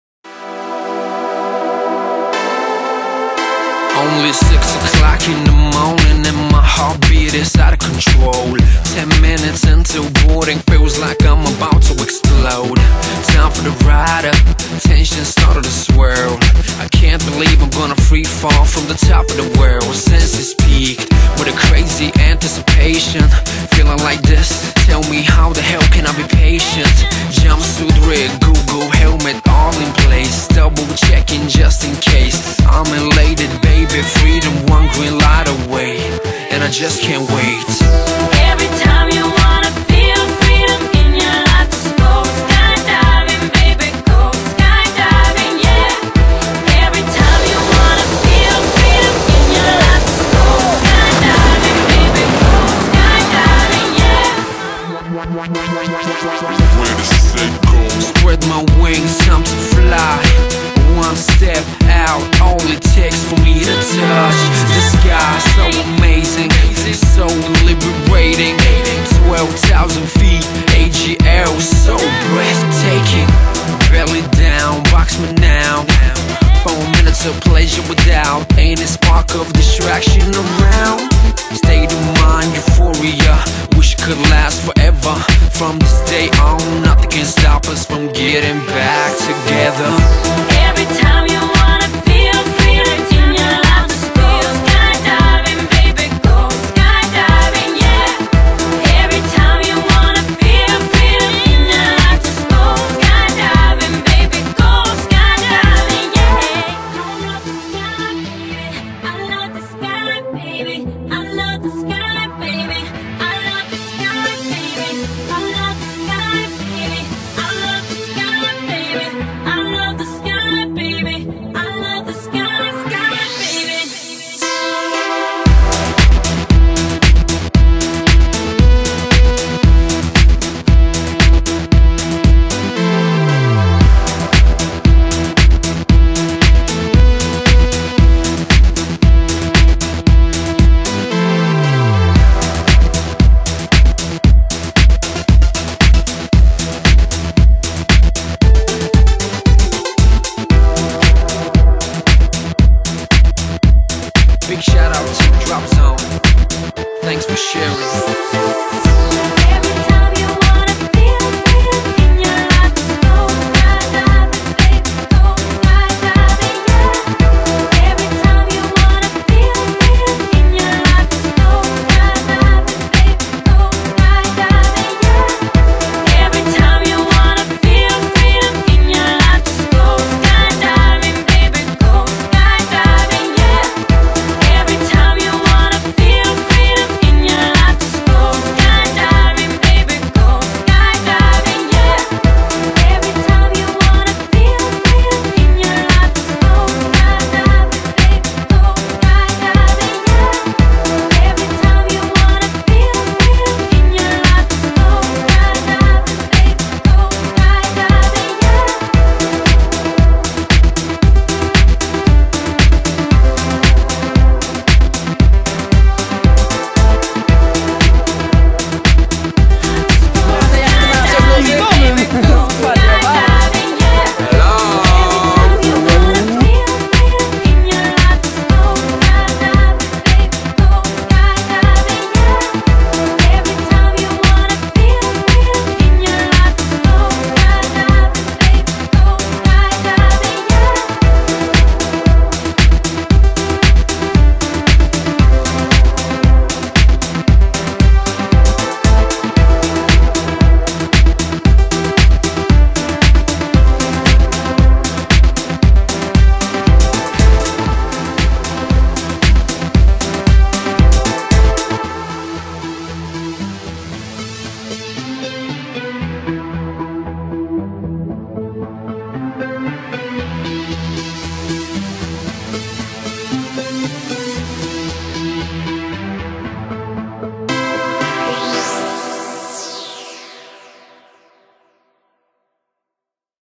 weekend 11 09
READY, SET. GO!!!
Słoneczny, pełny wspaniałych skoków wekeend w skydive.pl!